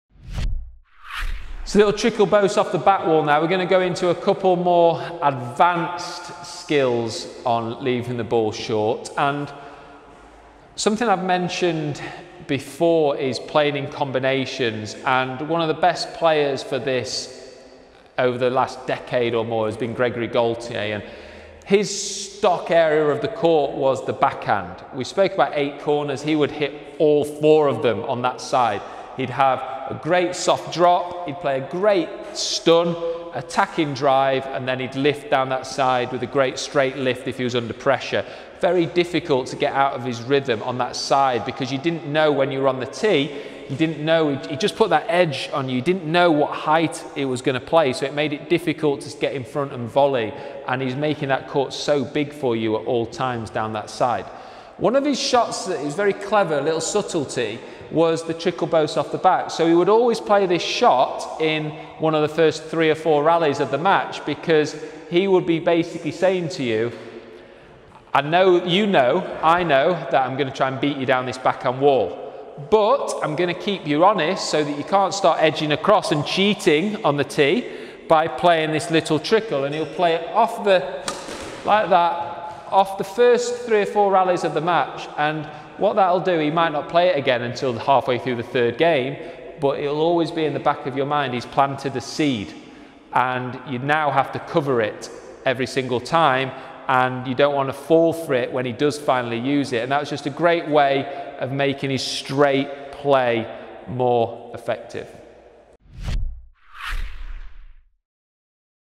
Squash Tips: Leaving The Ball Short With Nick Matthew - Playing In Combinations
In this video, Nick Matthew expands on how to play shots in combinations.

Specifically, Nick analyses Gregory Gaultier and his ability down the backhand wall to be able to play shots at any height at any time, expanding on how he was able to make the court extremely big for his opponent.